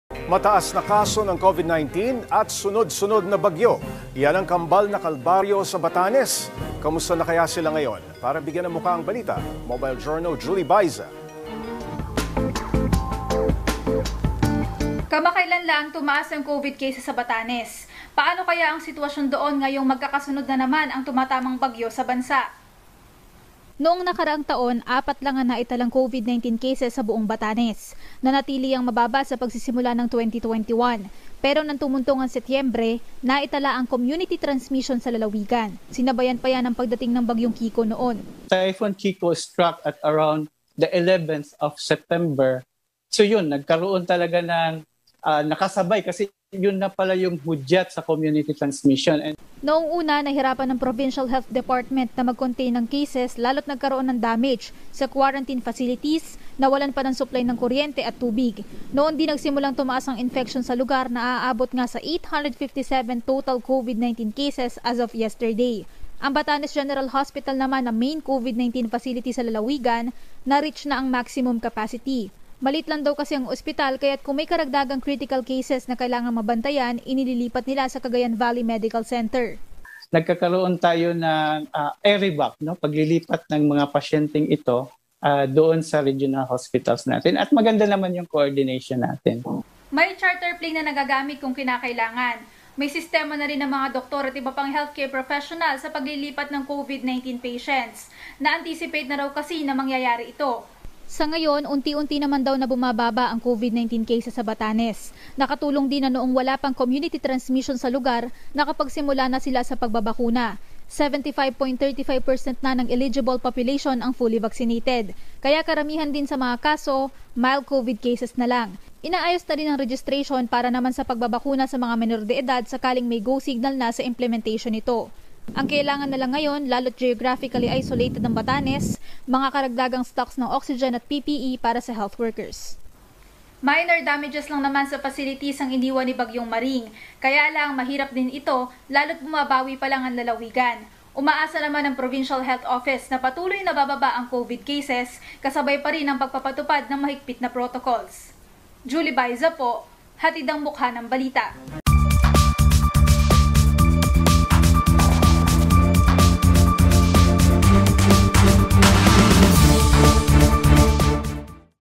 COVID-19 at bagyo sa Batanes
OneBalitaPilipinas | Naitala ang pinakamataas na community transmission ng COVID-19 sa Batanes matapos ang pananalasa ng bagyo noong Setyembre.

Kumusta na kaya ang sitwasyon sa lugar ngayong hinagupit muli sila ng bagyo? | via MOJO Julie Baiza